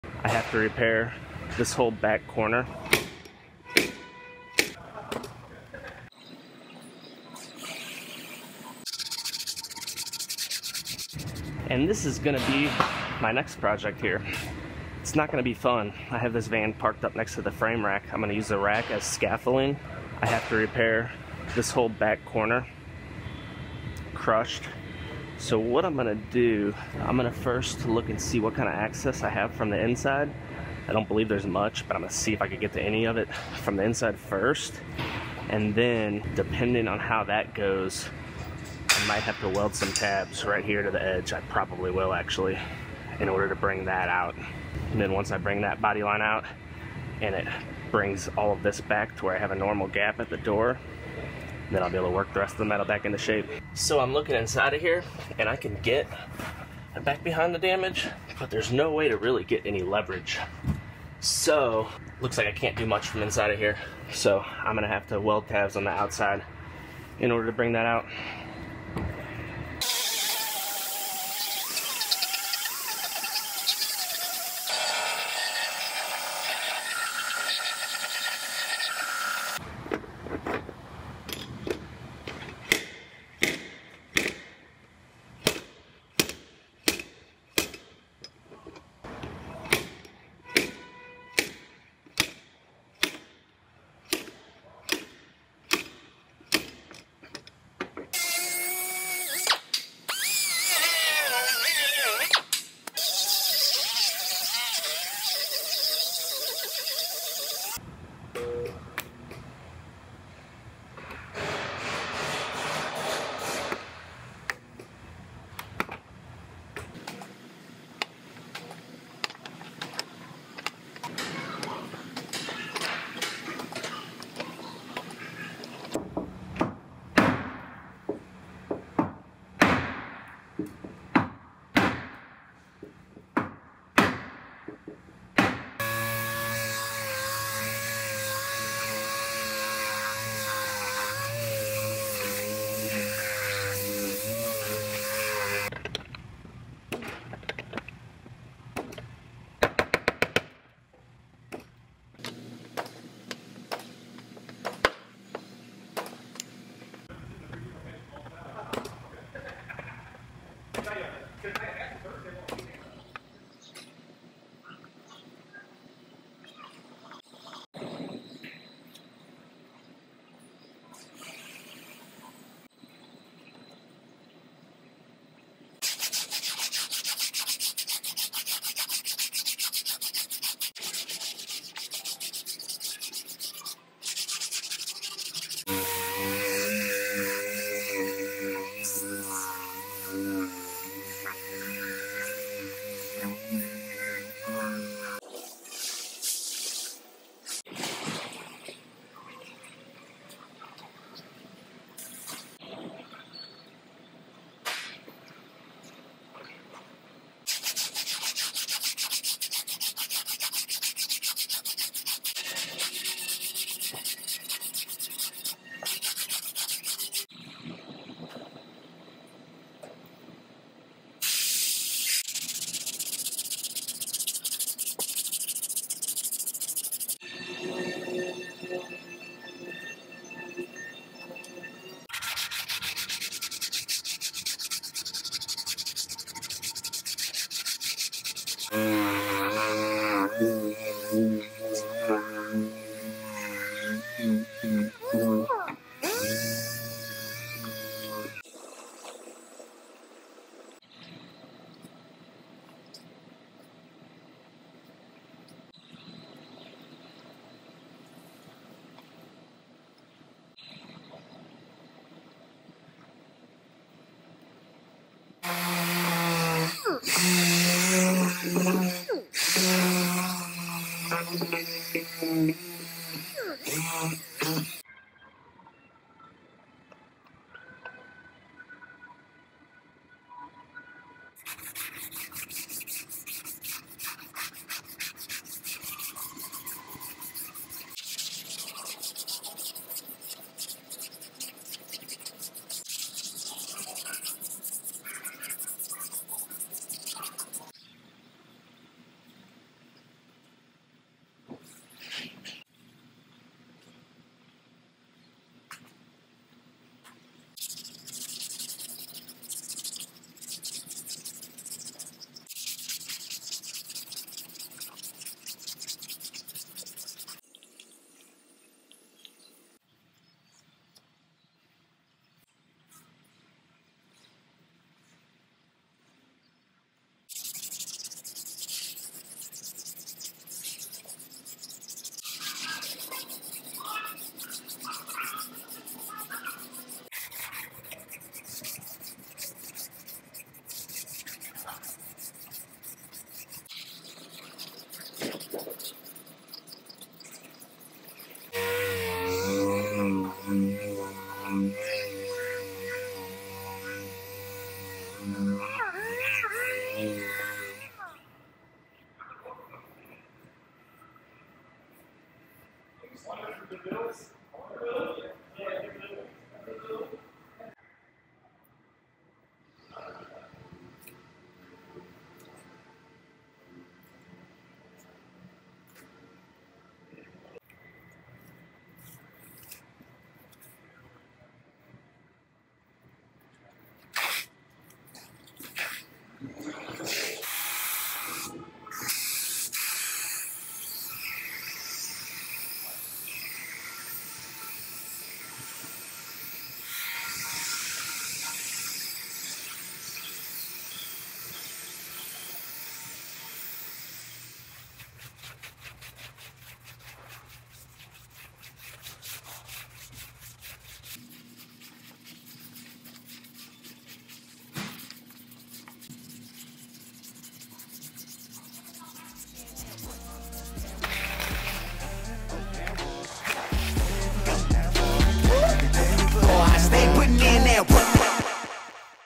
Workin' High AF!
Sometimes we have vehicles come in that are just not fun and this van is one of those vehicles!  Not only is it a pain to work on but the damage is at the very top of this tall ass van.  I parked it next to the frame machine and used it to stand on to do the repairs but working high af up in the air is still never fun!  It’s all part of the job though, gotta take the good with the bad!

Be sure to follow along for other content!